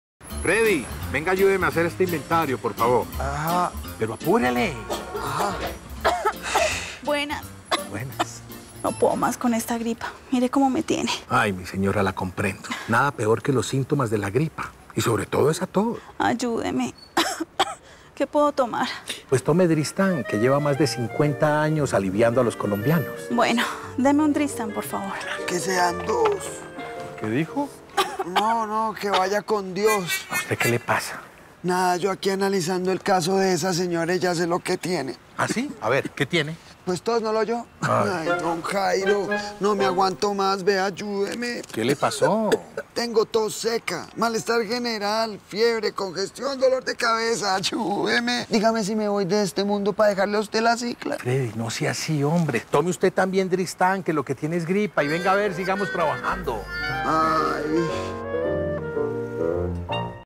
9. Dristan Short Gag Movies. Colombia Drugstore stories...
Our two main actors just are great!

12 sketches with a smile twist at the end.

Written and created by Diego Briceño, creative at PC Caracol.
Directed and photographed by Juan Carlos Cajiao